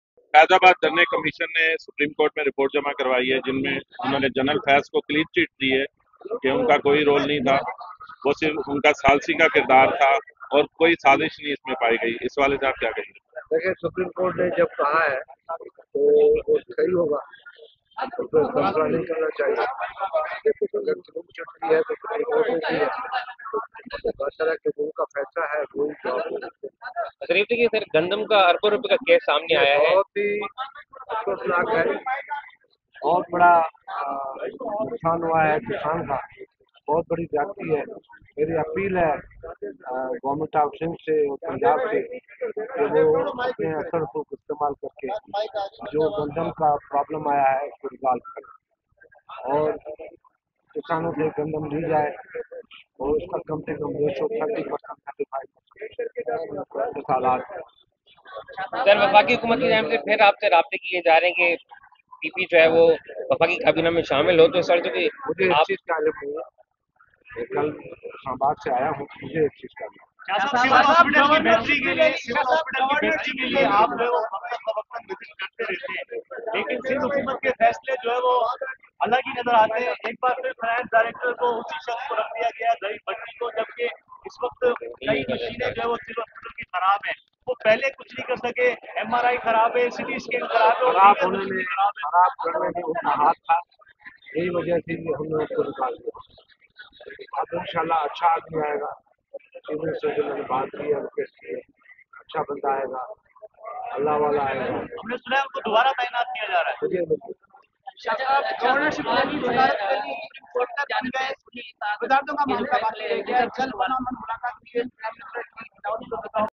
Khurshid Shah has no knowledge about joining the People's Party in the federal government 06 May 24
Khurshid Shah has no knowledge about joining the People's Party in the federal government 06 May 2024
‏پیپلزپارٹی کے وفاقی حکومت میں شمولیت بارے کوئی علم نہیں ، خورشید شاہ